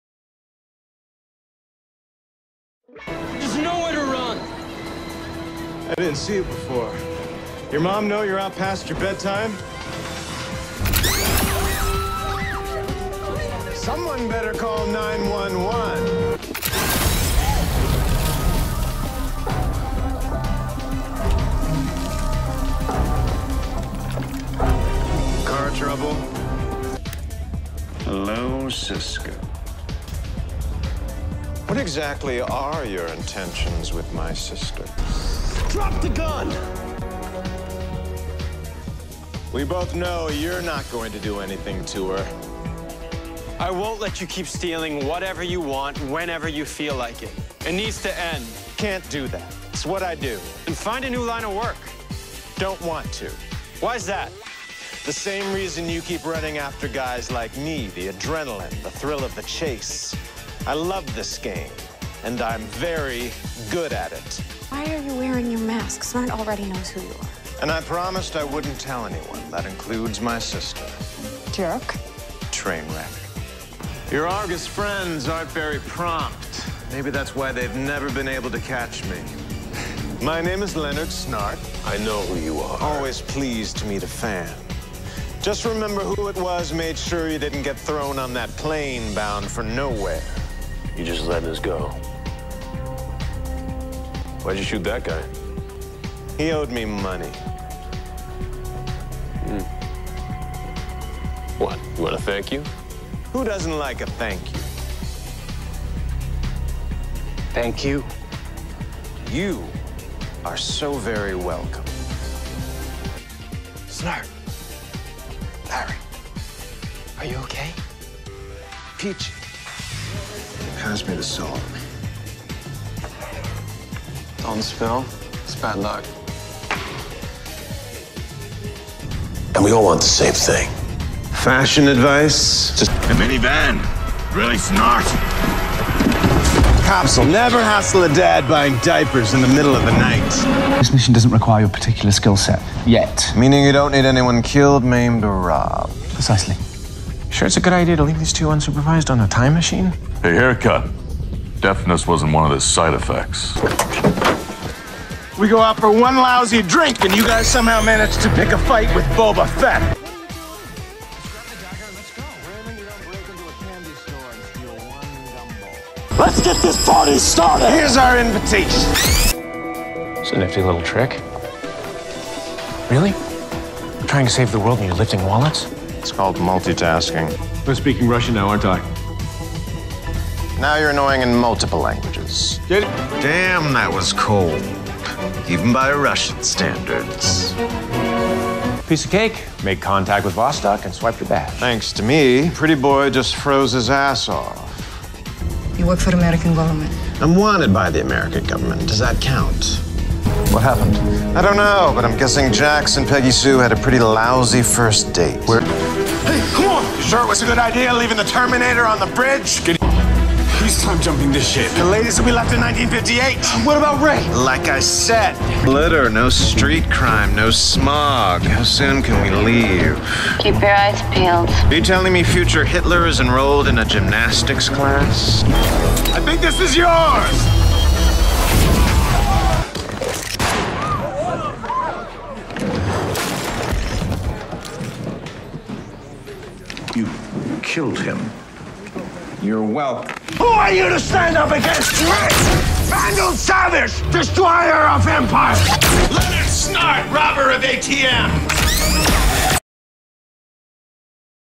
Leonard Snart || The Best of Snart's Snarky Snarkisms (Humor)
Collection of Fun, Sarcastic and humorous quotes by Leonard Snart (Captain Cold).